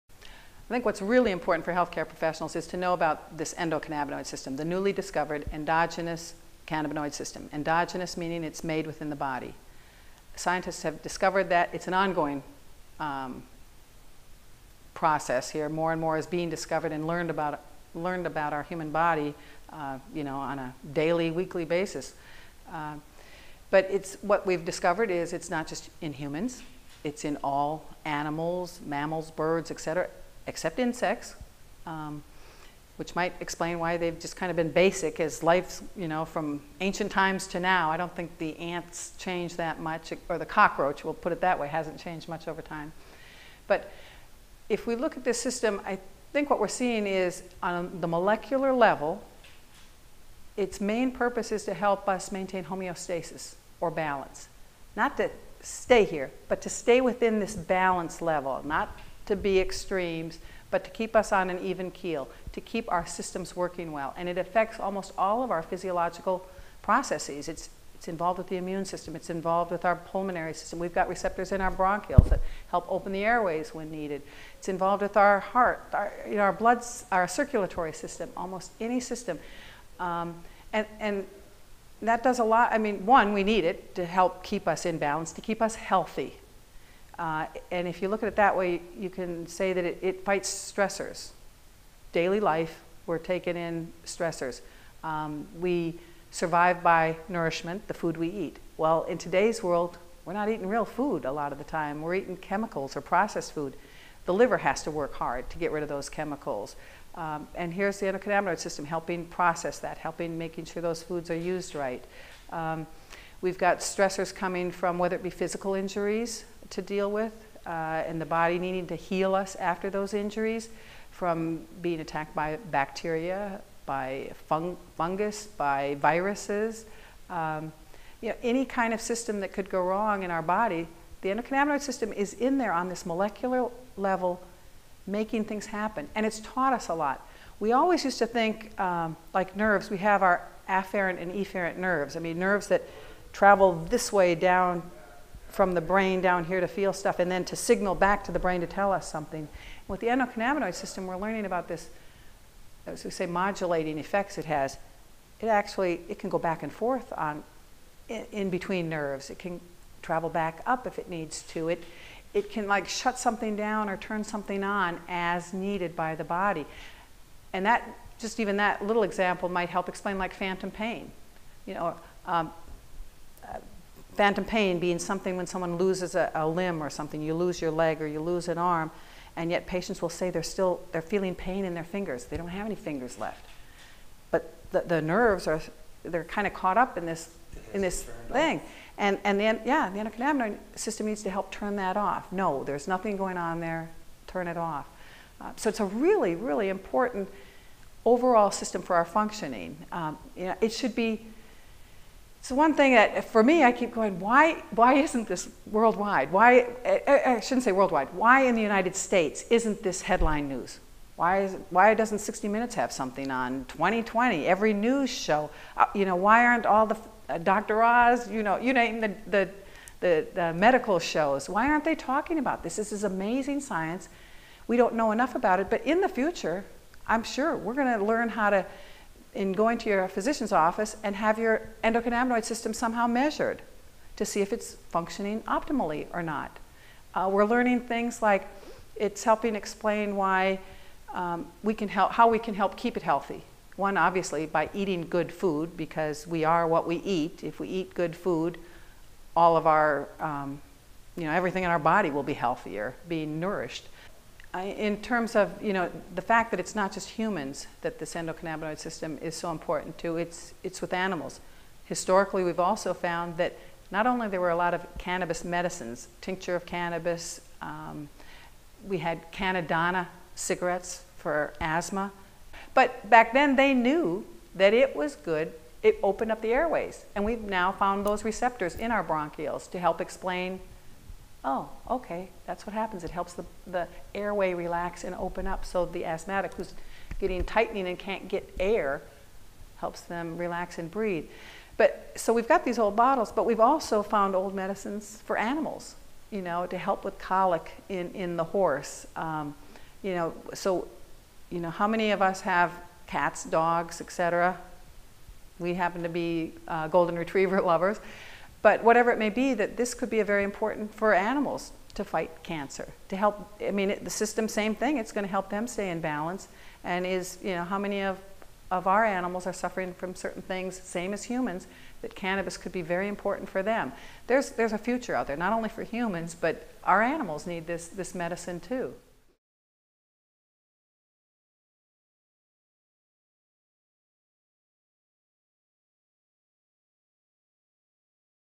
Mathre: The ECS System Part 2, Balance
In the second part of this series, Mary Lynn Mathre discusses the primary role of the Endocannabinoid System, homeostasis or "Balance".   Science has shown that this system not only exists in humans, but in all mammals, regulating virtually every function.
For more information about the Endocannabinoid System, visit our Patients Out of Time ) website.